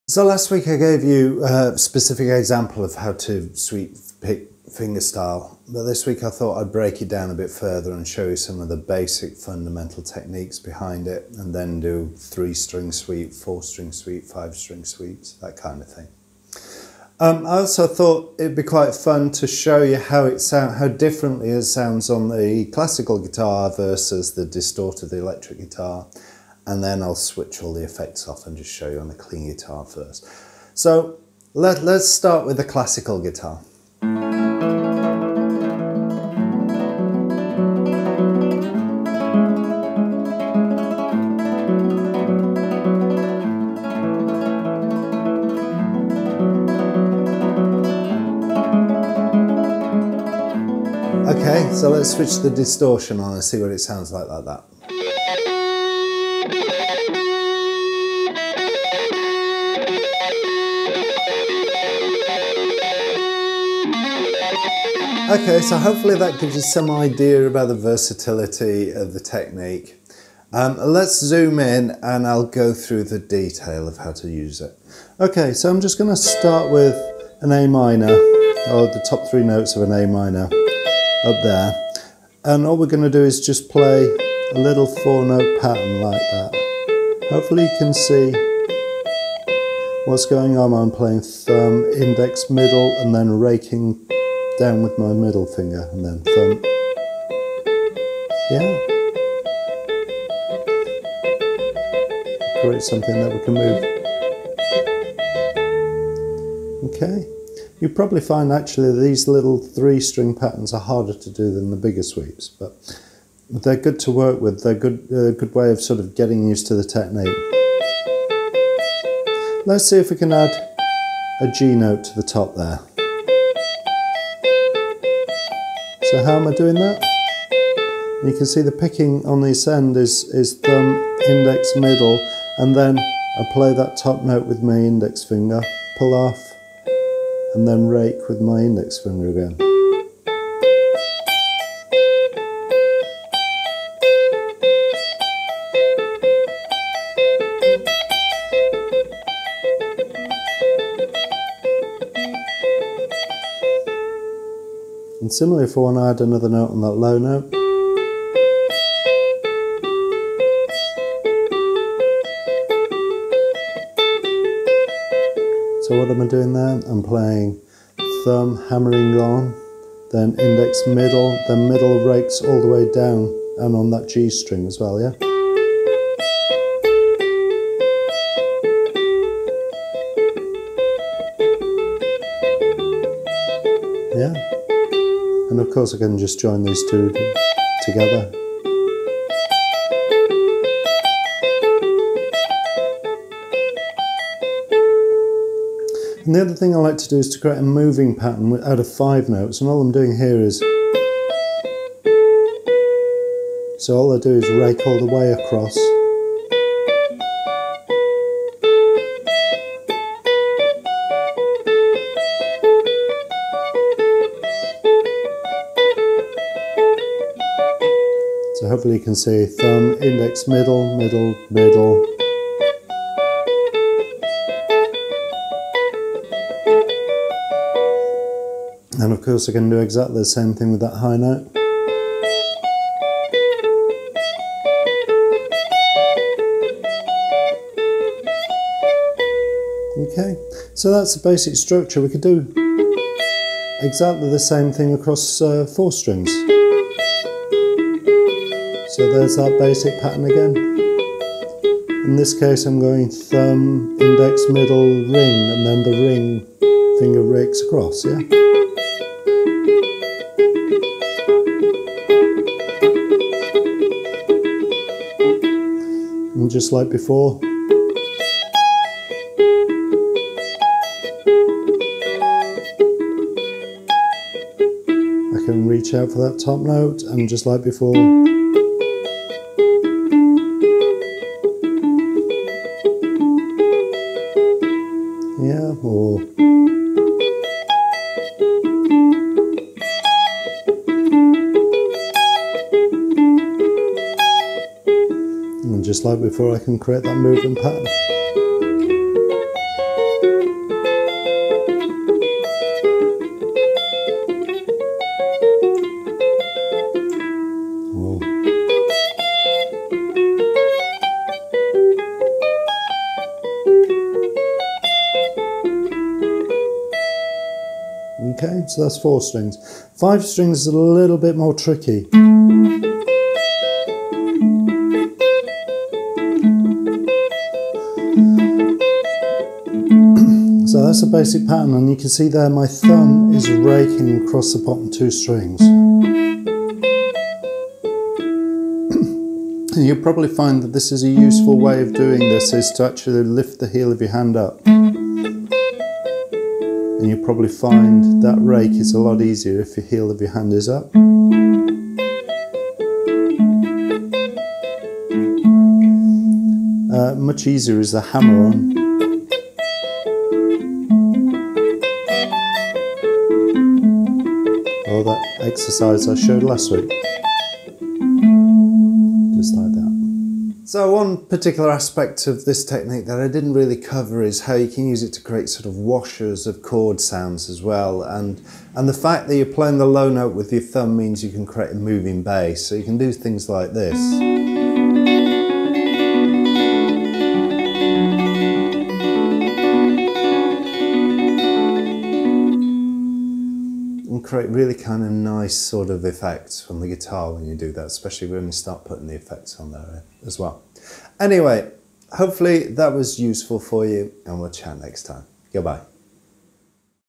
How to Sweep Pick with your Fingers
This week I thought I'd break down the fingerstyle sweep picking technique further and demonstrate a set of 3, 4, and 5 string variations.

I also thought it would be fun to show how this same technique sounds on the classical guitar and distorted electric guitar too.